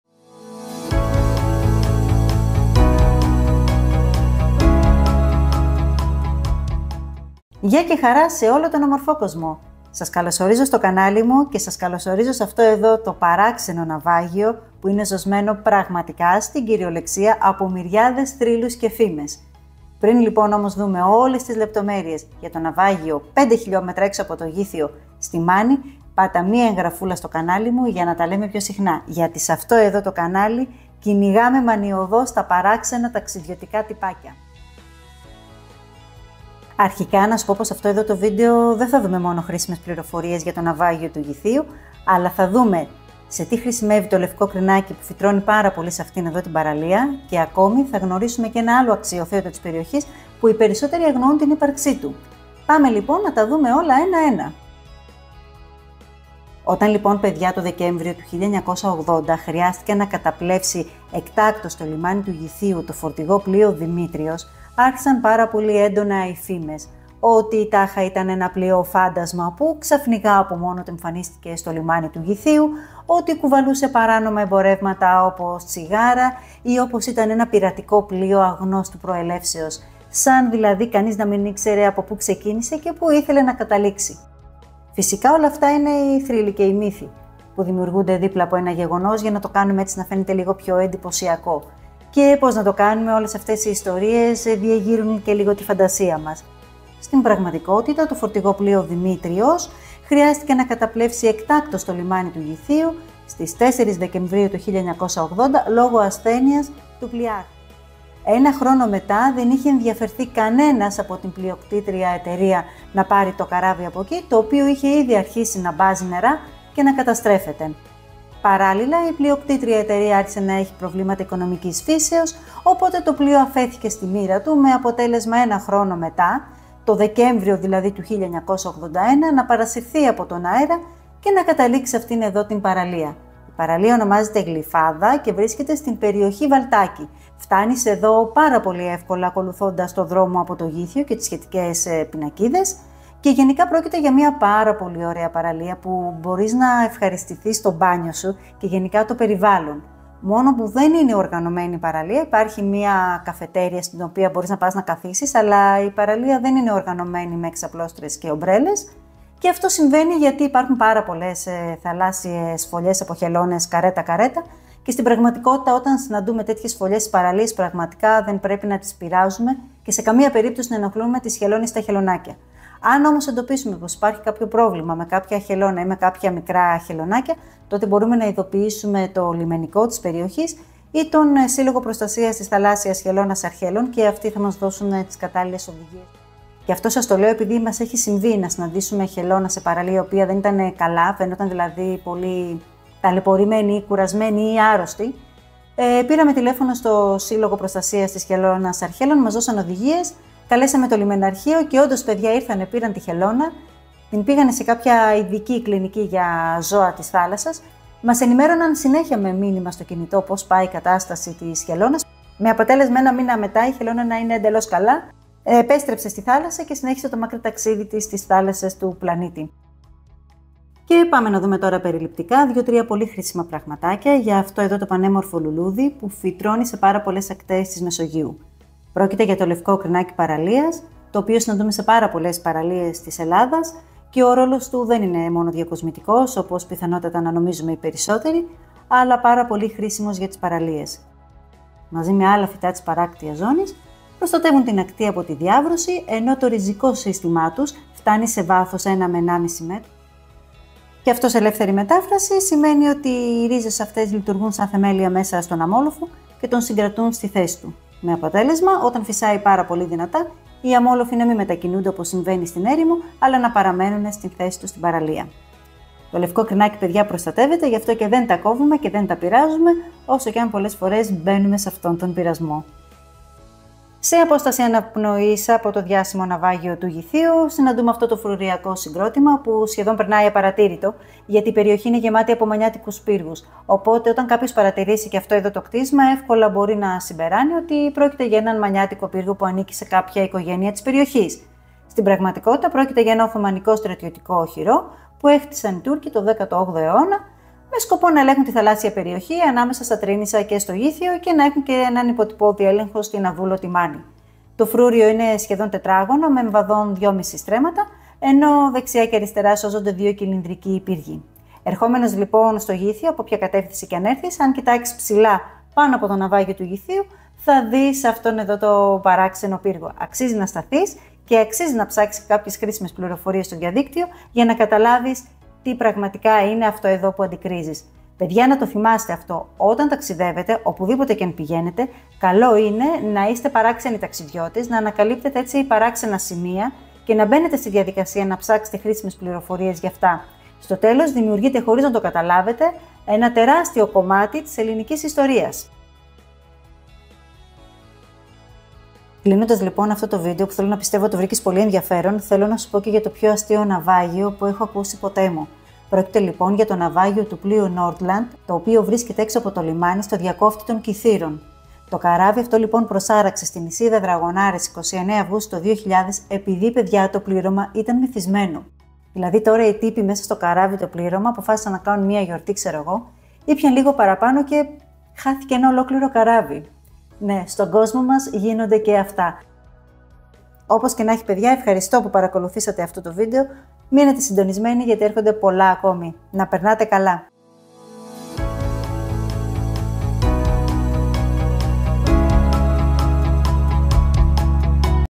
Το διάσημο καράβι στο Γύθειο Λακωνίας και άλλα tips για την παραλία
Το ναυάγιο Δημήτριος στο Γύθειο Λακωνίας,  βρίσκεται στην παραλία Γλυφάδα από το Δεκέμβριο του 1981 κι έκτοτε αποτελεί κορυφαία τουριστική ατραξιόν της περιοχής.

Ζωσμένο από θρύλους και πειρατικές ιστορίες το κουφάρι του λακωνικού κόλπου, προσελκύει κάθε χρόνο χιλιάδες επισκέπτες. Πλέον θεωρείται ένα από τα πιο φημισμένα ναυάγια της Ελλάδας και μετά από 40 (και πλέον χρόνια), απόλυτα εναρμονισμένο με το φυσικό τοπίο.

Ποια είναι όμως η αλήθεια πίσω από τους θρύλους που γιγαντώνουν τη φήμη του; Και ποιο είναι αυτό το σπάνιο λουλούδι που φυτρώνει μόνο σε παραλίες; Κι αυτό το φρούριο, τι δουλειά έχιε πάνω από το ναυάγιο;

Δες όλο το βίντεο και πες μου στα σχόλια, πόσα από αυτά γνώριζες!

Δες εδώ άρθρο (με χαρτί και καμάρι δηλαδή) αν βαριέσαι να με ακούς